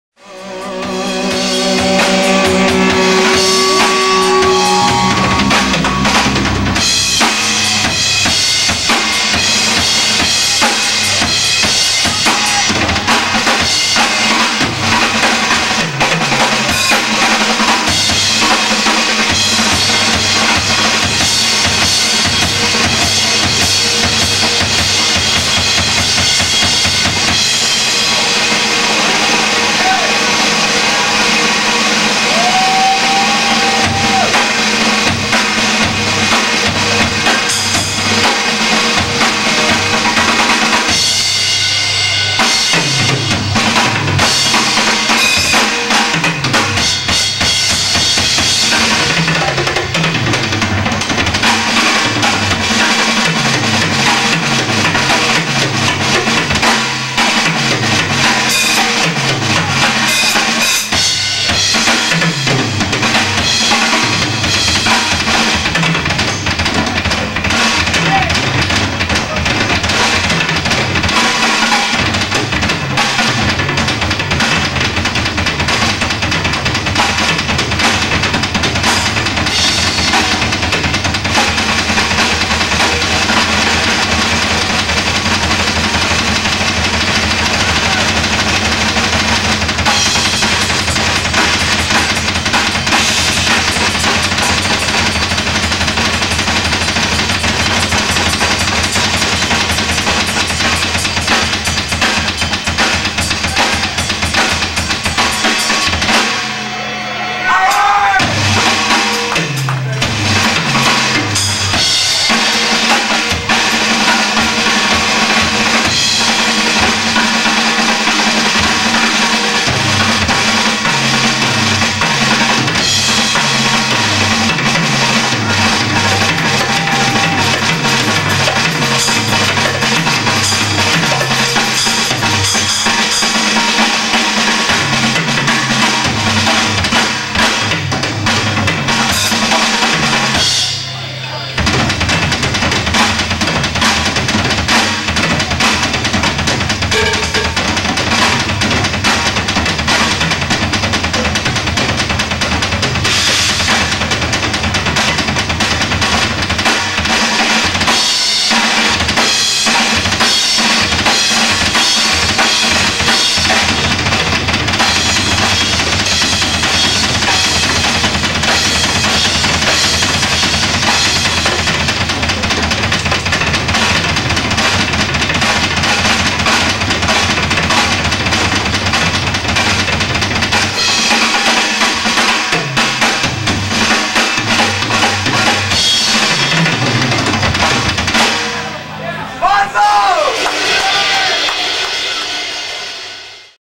Club Ruins Solo
Here's a video I made back in 1993 in my rockin' days when I was in Laughing Sam's Dice. The kit is vintage 1982 Tama Imperialstar in metallic white. The toms are: 8", 10", 12", 14" tom toms, & a 16" floor tom. The bass drums are 22" & 24", and the snare is a 14" Pearl Free Floating Piccalo with a maple shell. The cymbals are a mix of Ziljian, Paiste, Profile & Wuhan: 20" Zildjian Ping Ride, the cable hat has a 18" Zildjian Med/Thin Crash on the bottom with an 18" Profile Fast Dragon Crash on the top. 18" Wuhan China Type, 17" Paiste Signature Full Crash, 18" Paiste 2002 Crash, 8" Paiste Signature Splash. The hi-hats are 14" Zildjian Quick Beats. All the hardware is Tama Titan. The heads I was using at the time were Remo black dot Ambassadors on top with Remo Ebony Ambassadors on the bottom.